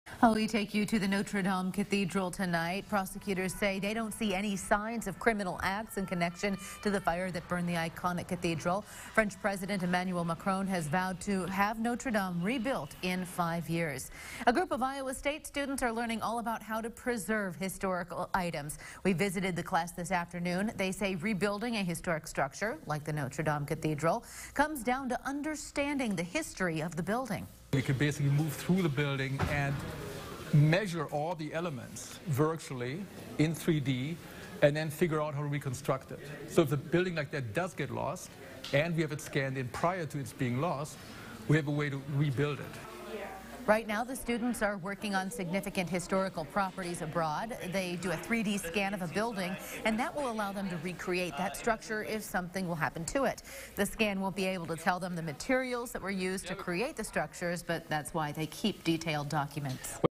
ISU students learn how to preserve historical items.
Students say rebuilding a structure like the Notre Dame Cathedral comes down to understanding the history behind it.